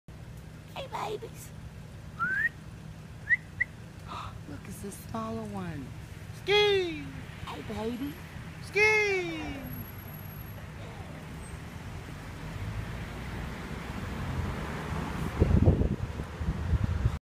Two Wild Bears in Cherokee NC 8-5-14
Two Wild Bears 5 foot from us near the Road!!!